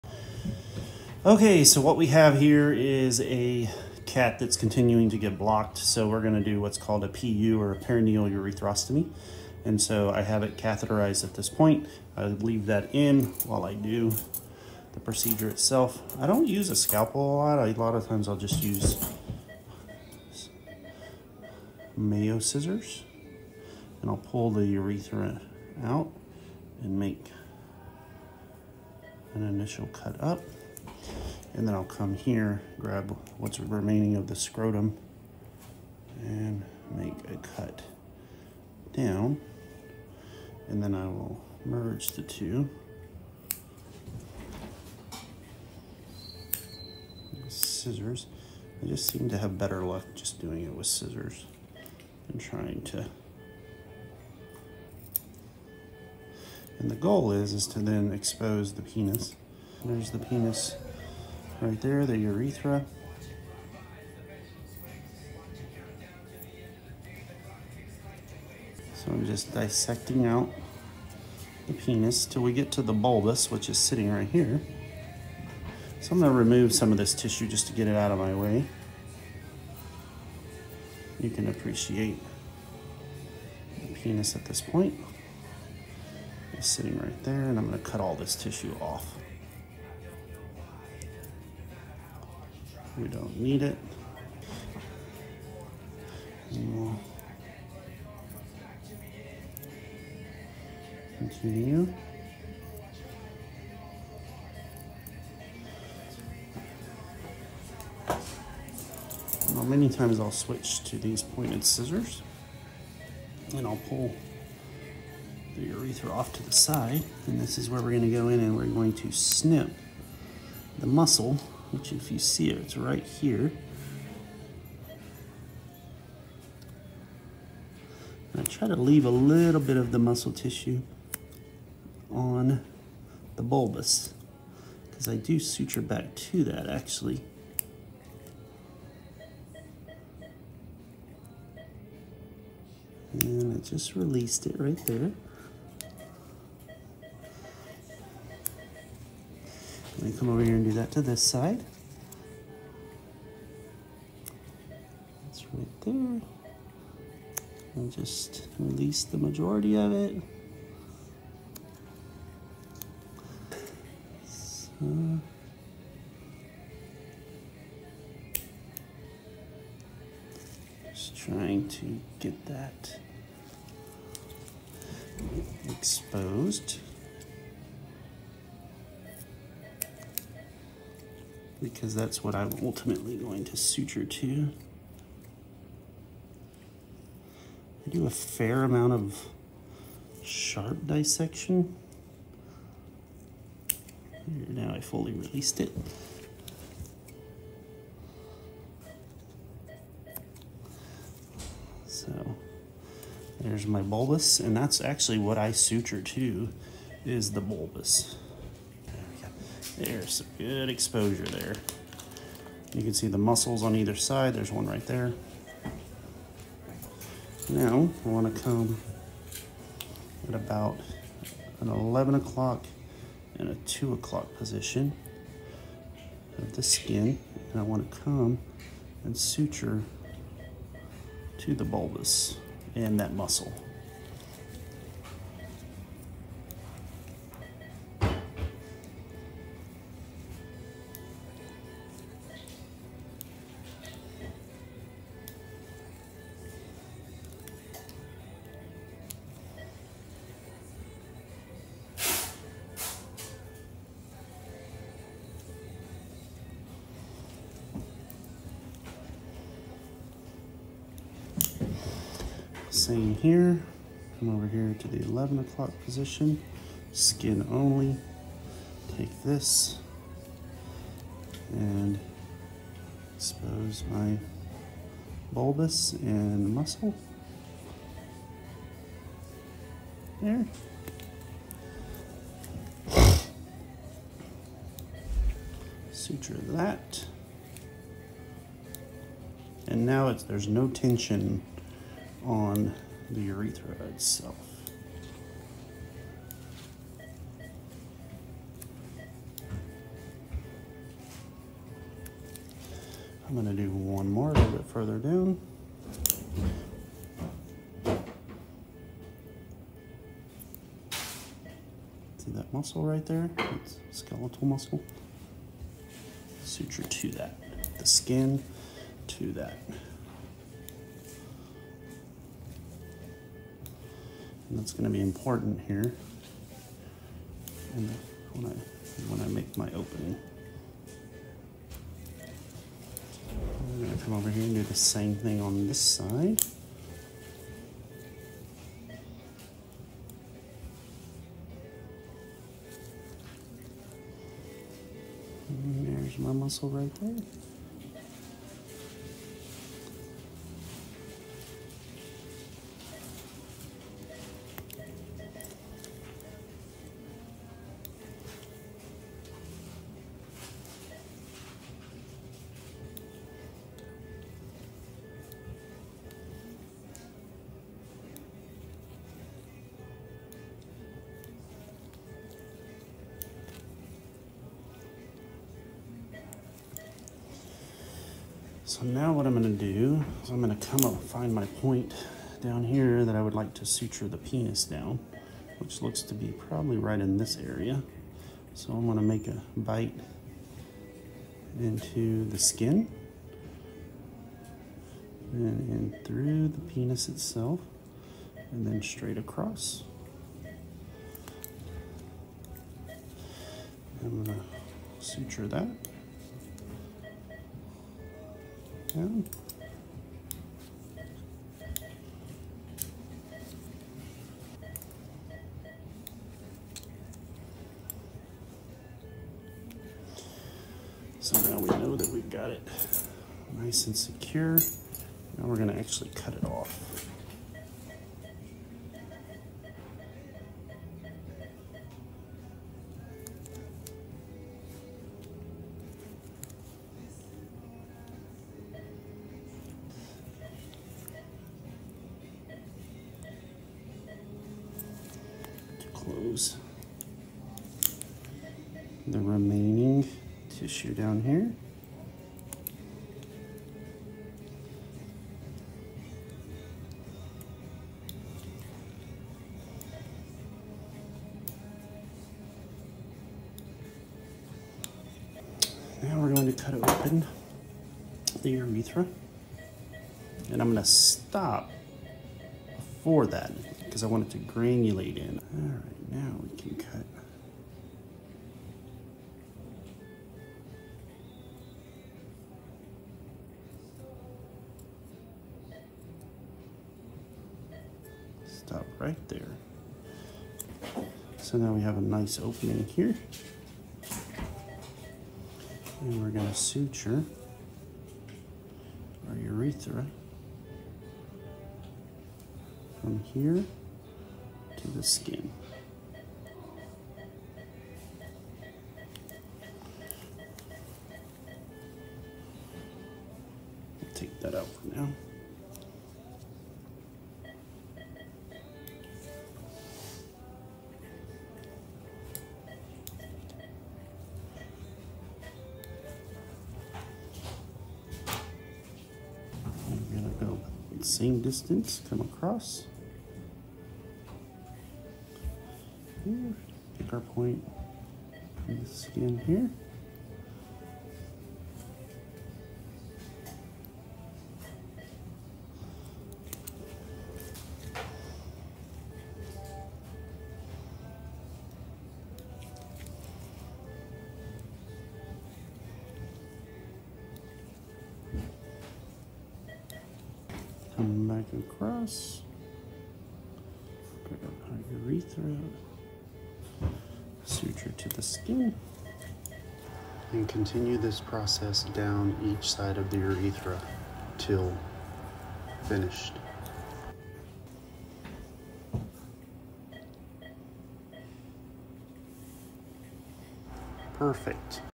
The Blocked Tom - Male Feline Perineal Urethrostomy Surgery PU, Edited Version
In this video, Dr. Mark Besançon, DVM, performs a perineal urethrostomy (PU) surgery, a definitive procedure for male cats with recurrent or unresolvable urethral obstructions due to urethral plugs, strictures, or urolithiasis. The step-by-step surgical approach includes patient positioning, incision technique, urethral dissection, stoma creation, and post-op care considerations. Designed for veterinarians seeking to refine their surgical skills and improve outcomes in managing feline lower urinary tract disease (FLUTD), this video offers practical tips and real-time insights from the surgical field.